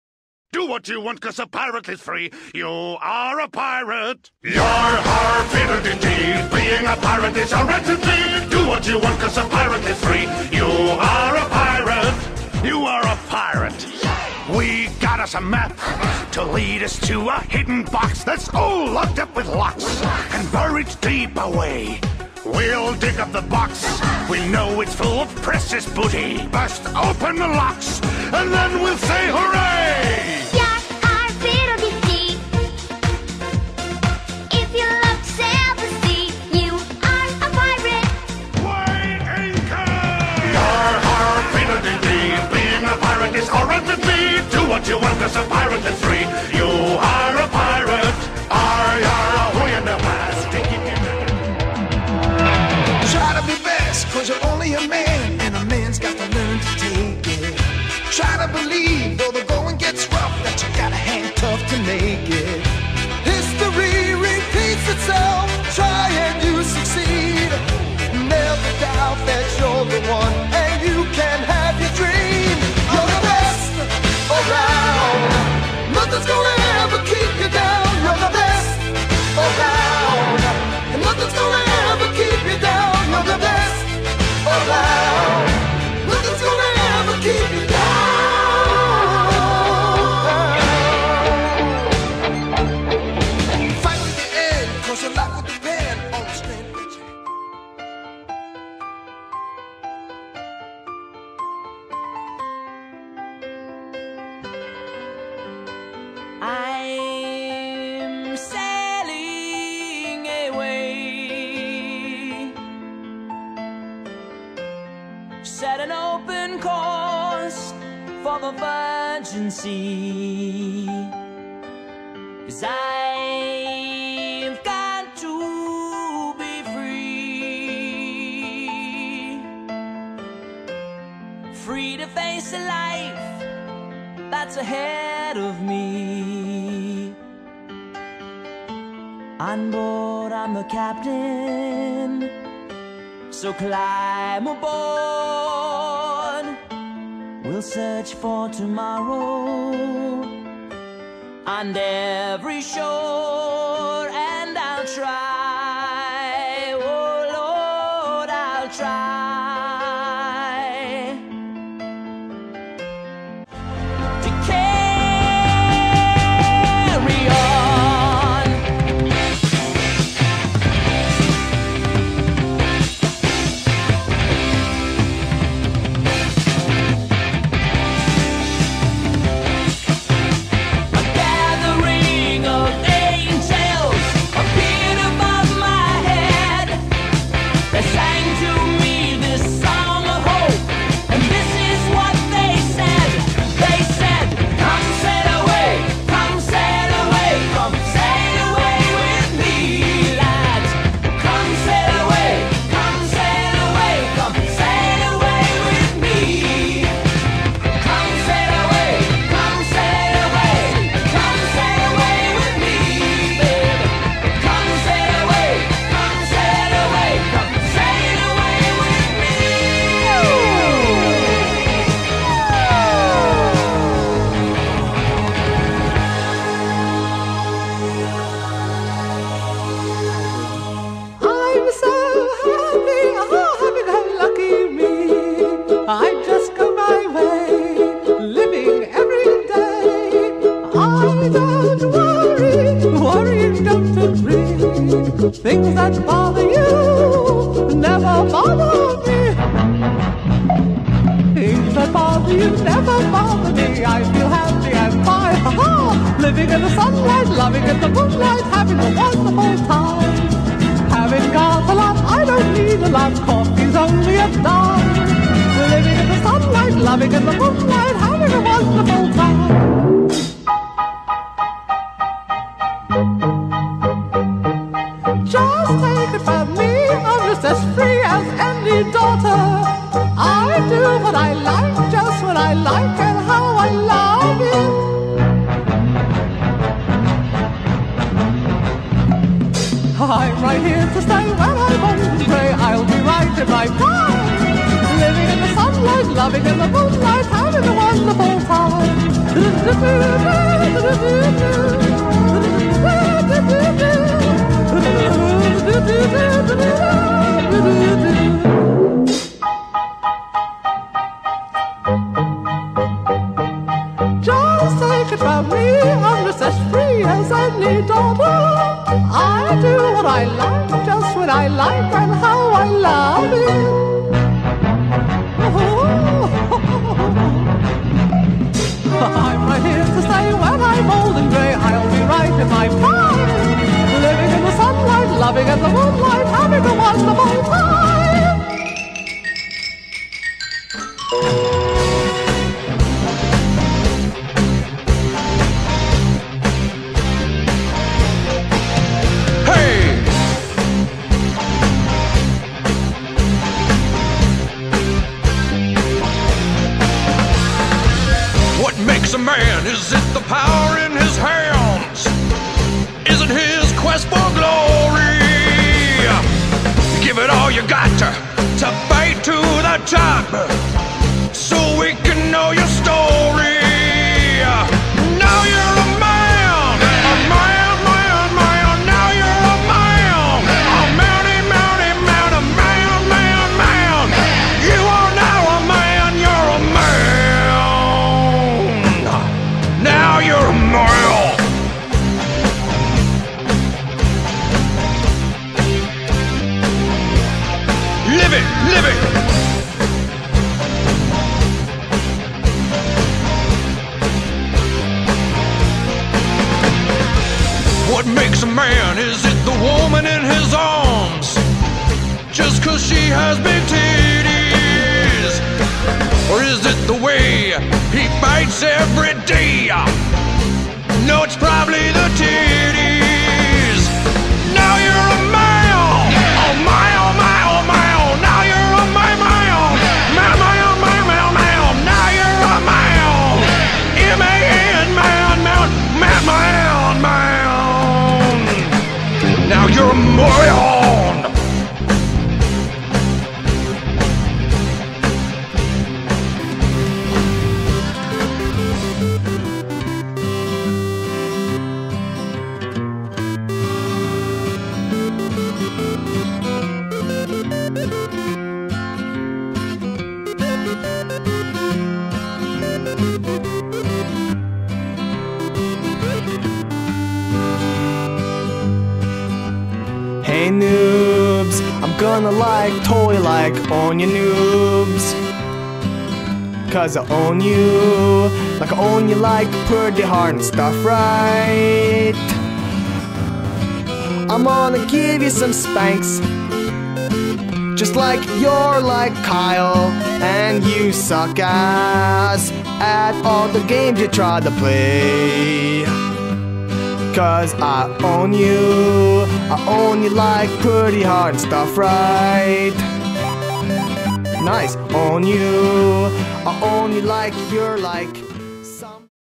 NFC (and old Cynical) Pirate Raid!
When we were in Cynical, some late night we all got drunk, turned into pirates, and went on an adventure. Hope you all enjoy!

John N. Productions